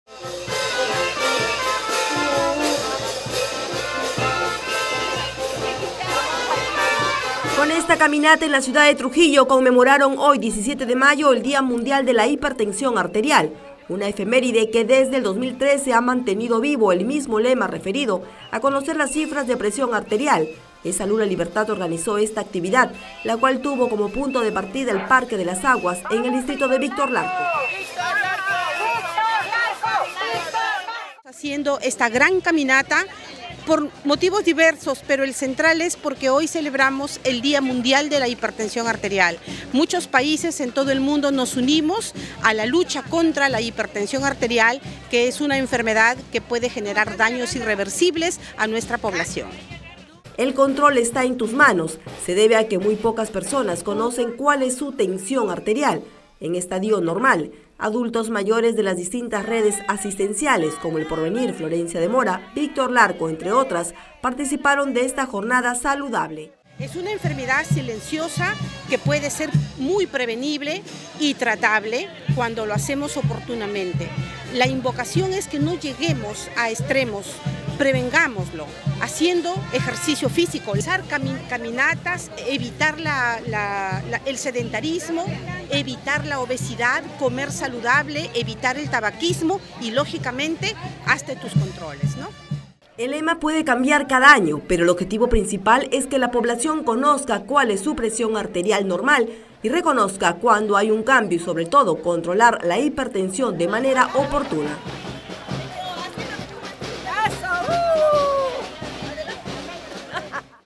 Víctor Larco: Organizan caminata por el día mundial de la hipertensión arterial
En el día mundial de la hipertensión arterial, EsSalud de La libertad organizó una caminata denominada “El control está en tus manos”, con la finalidad de que las personas conozcan su estadio normal y así evitar complicaciones.

Con esta caminata en la ciudad de Trujillo, conmemoraron el día mundial de la hipertensión arterial, una efeméride que desde el 2013 ha mantenido vivo el mismo lema referido a conocer las cifras de presión arterial.

EsSalud La Libertad, organizó esta actividad, la cual tuvo como punto de partida el parque de las aguas en el distrito de Víctor Larco.

 “El control está en tus manos" se debe a que muy pocas personas conocen cuál es su tensión arterial en estadio normal. Adultos mayores de las distintas redes asistenciales como El Porvenir, Florencia de Mora, Víctor Larco, entre otras, participaron de esta jornada saludable.

Este lema puede cambiar cada año, pero el objetivo principal es que la población conozca cuál es su presión arterial normal y reconozca cuando hay un cambio, sobre todo controlar la hipertensión de manera oportuna.